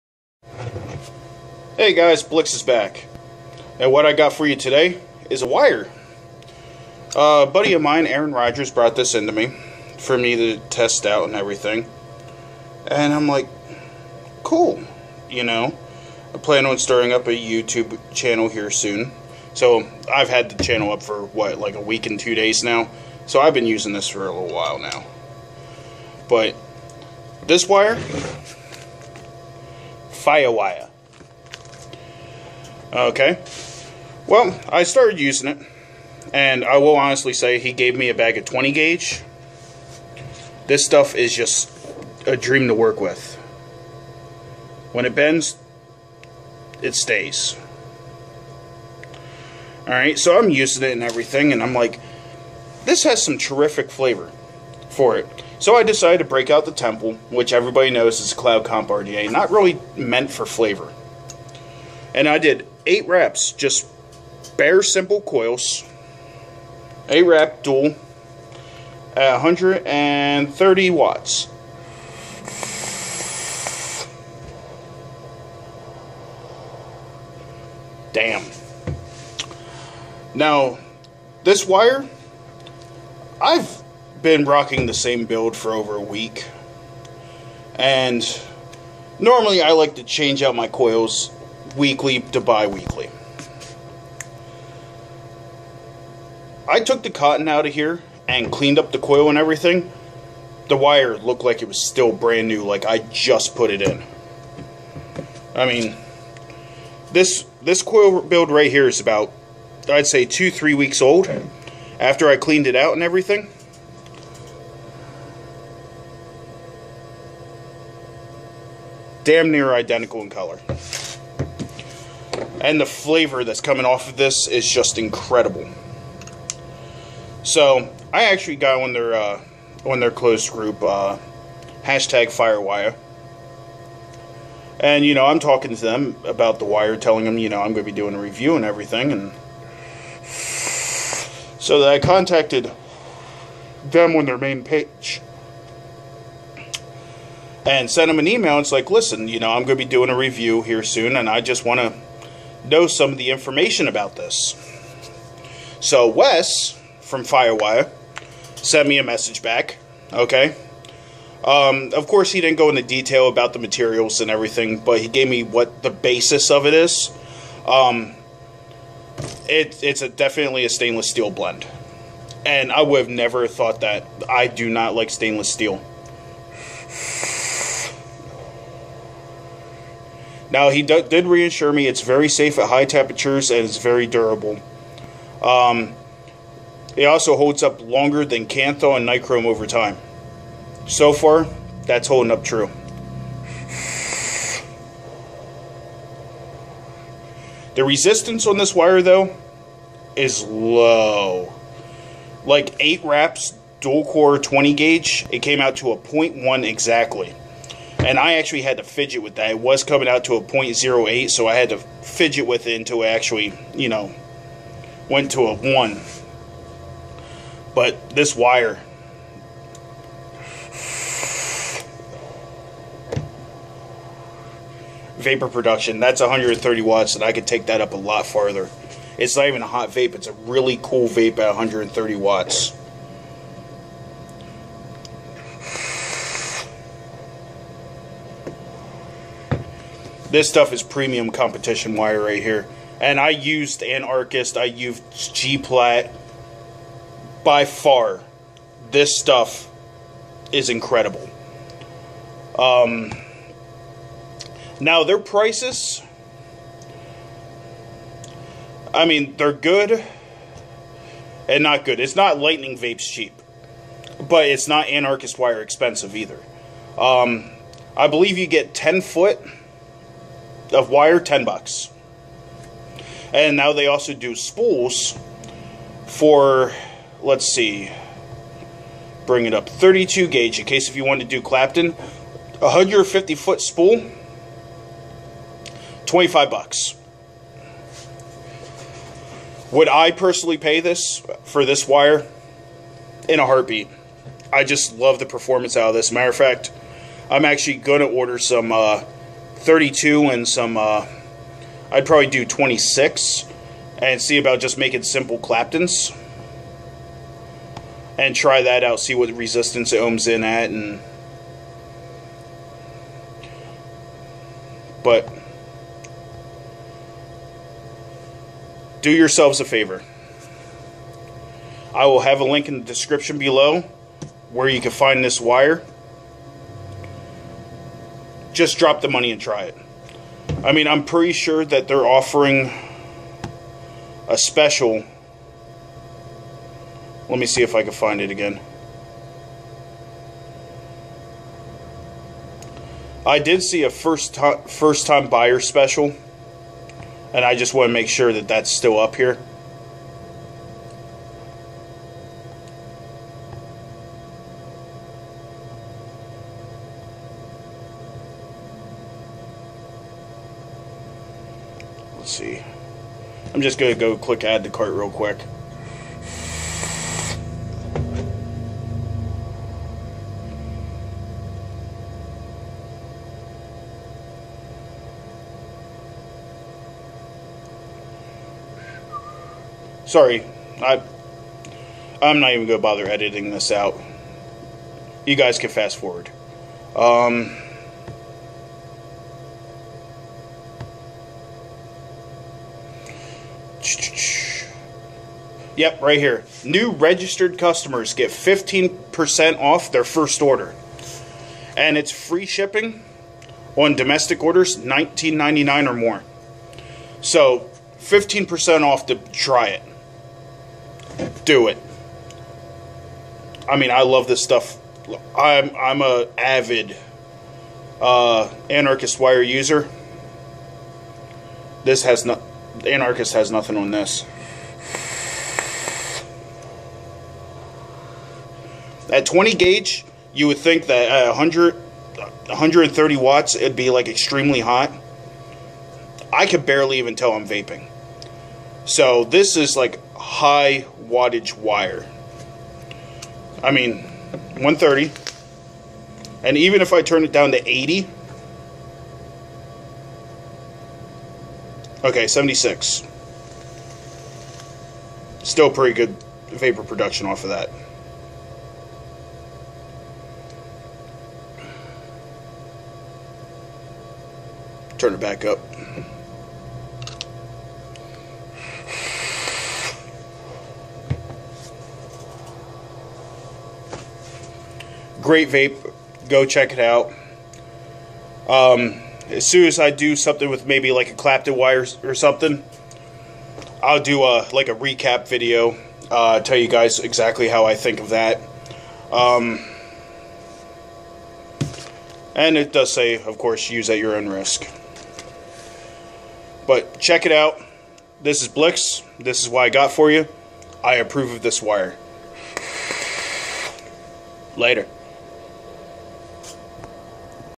FIYAH WIYAH
Today we look at a new wire FIYAH WIYAH. This stuff is incredible! Big shout out to Aaron Roger for hooking me up with this wire. And a big shout out to Wes at FIYAH WIYAH for answering my questions and being very quick emailing me back.